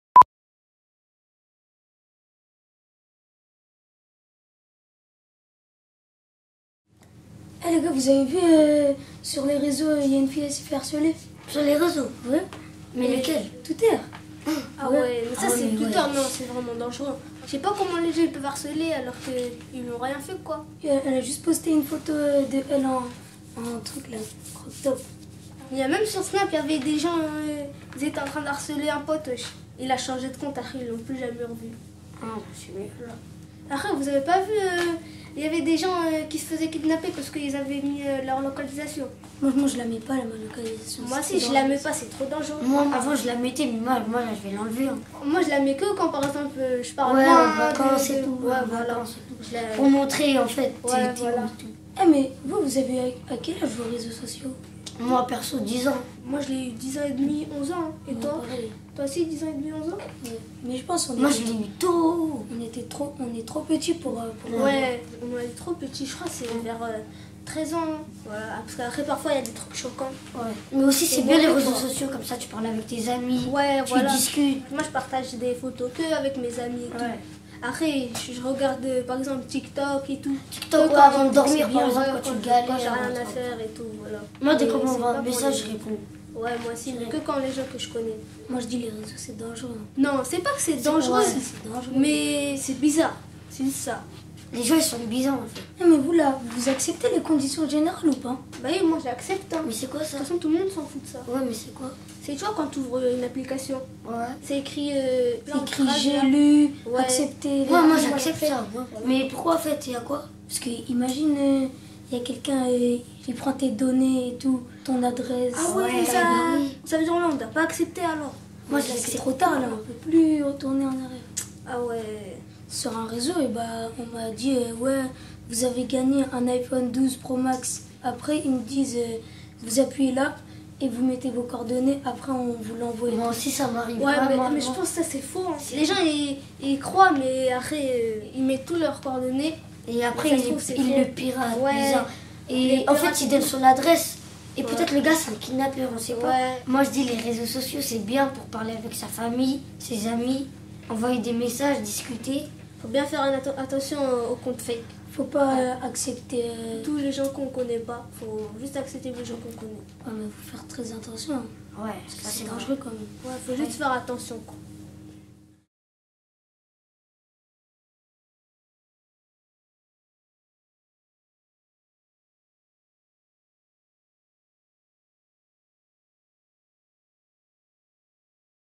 TCHATCH' #05 💬 | LES RÉSEAUX SOCIAUX
Cette série a été réalisée dans le cadre des activités proposées par l'Association L'Agora, le Centre Social AL2E à Évreux, avec la participation du Centre Social de La Madeleine.

« TCHATCH’ » est une série de 9 épisodes. Dans chaque épisode, différents groupes de jeunes ont choisi d’aborder selon leurs envies des thèmes d’actualité sur lesquels ils souhaitaient débattre (télé-réalité, réseaux sociaux, le monde en 2050, l’amour, l’amitié fille-garçon…etc). Chaque épisode est une immersion au cœur de leurs échanges, de leurs réflexions et de leurs préoccupations… tout cela sans filtres, avec bienveillance et humour! 😉